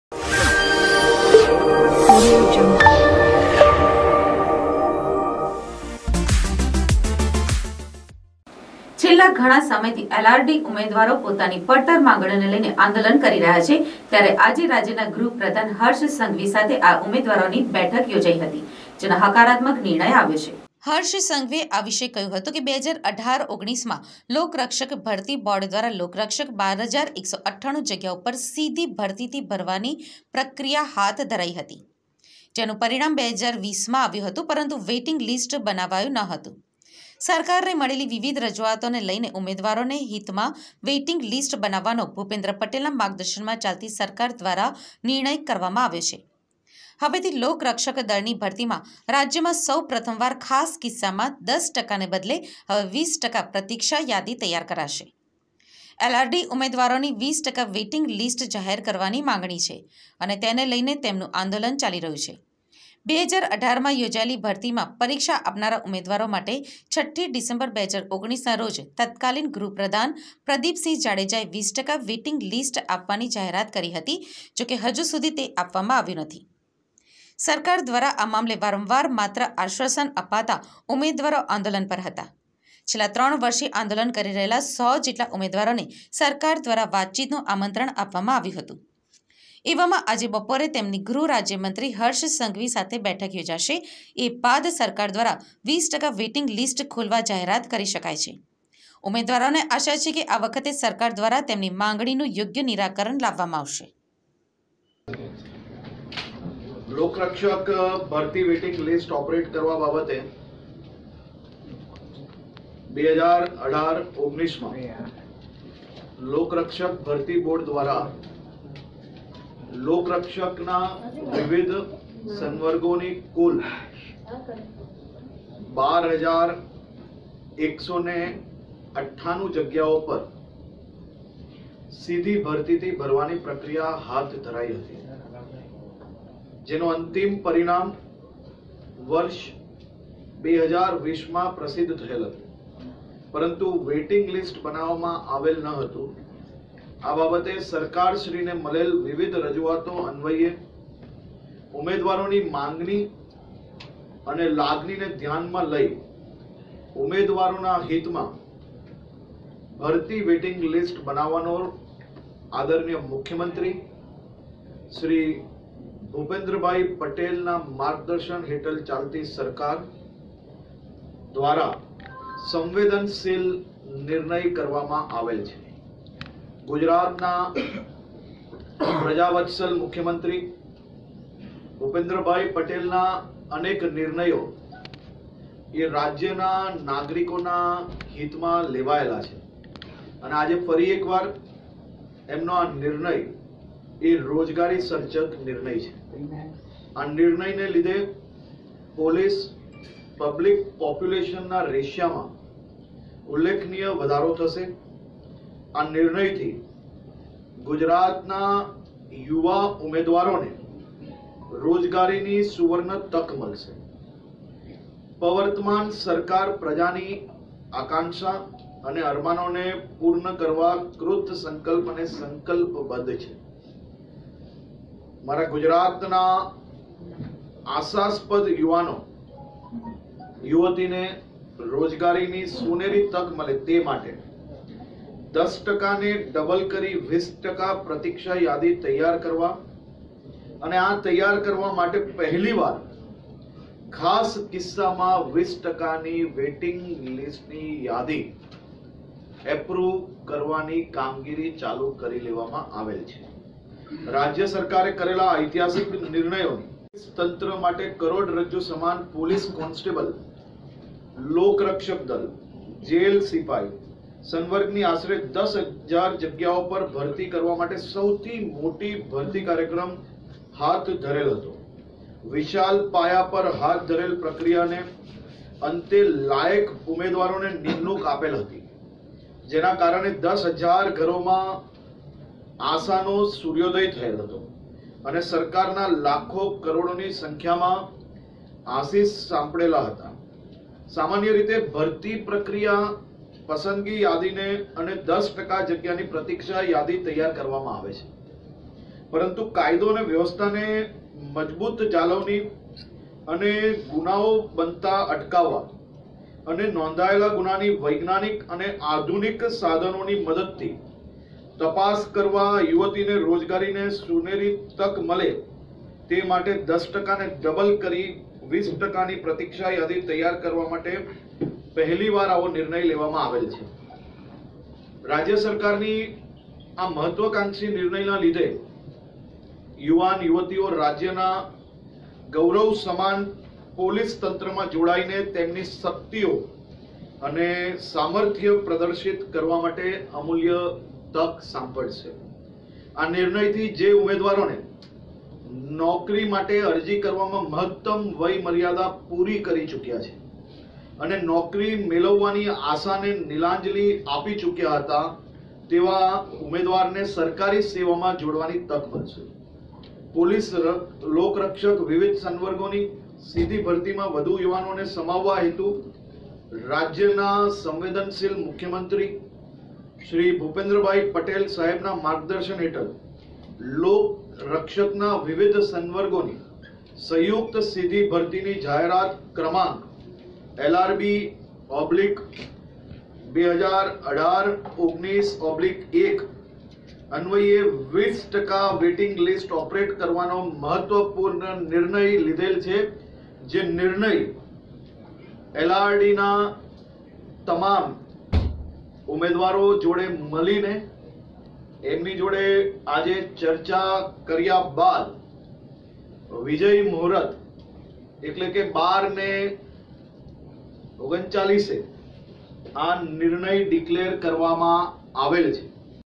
LRD આંદોલનનો અંત, ગુજરાત સરકારે 20% વેઈટિંગ લિસ્ટ સહિતની માગો સ્વીકારી
છેલ્લા ઘણા સમયથી LRD ઉમેદવારો પોતાની પડતર માંગણીઓને લઈને આંદોલન કરી રહ્યા છે. ત્યારે આજે રાજ્યના ગૃહપ્રધાન હર્ષ સંઘવી સાથે આ ઉમેદવારોની બેઠક યોજાઈ હતી,